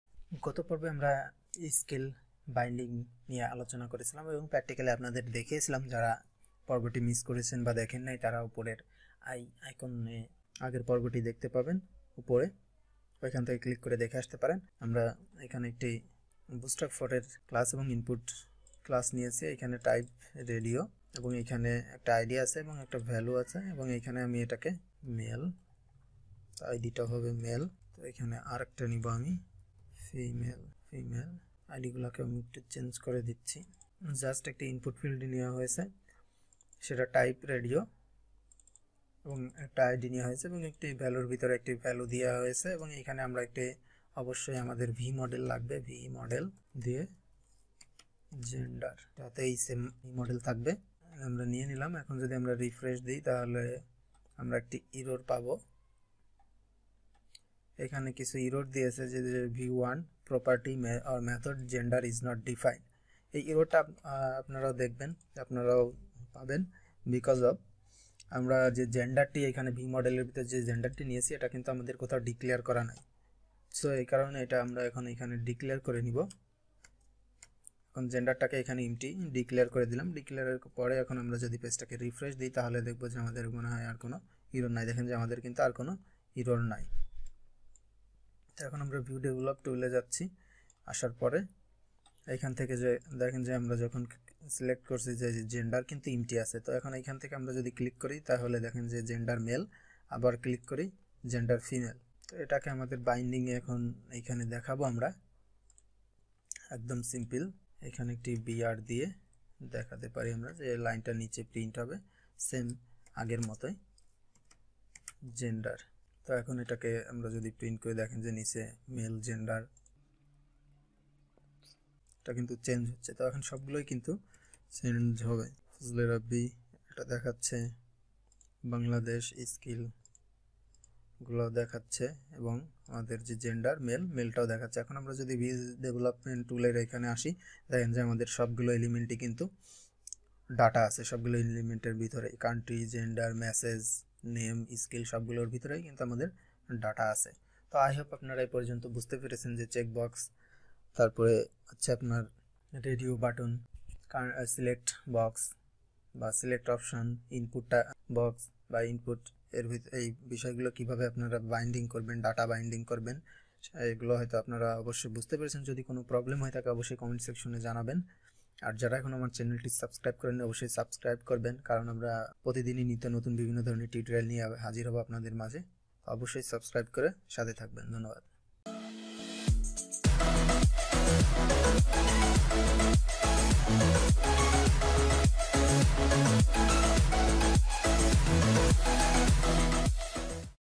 Vue JS Bangla Tutorial - Radio Button Data Binding Vue js Part 6 - 2019 by Coding Xpress
Student Management System Project - Laravel 5.7

Check box data binding vue js tutorial
vue js bangla tutorial
bangla vue js tutorial 2019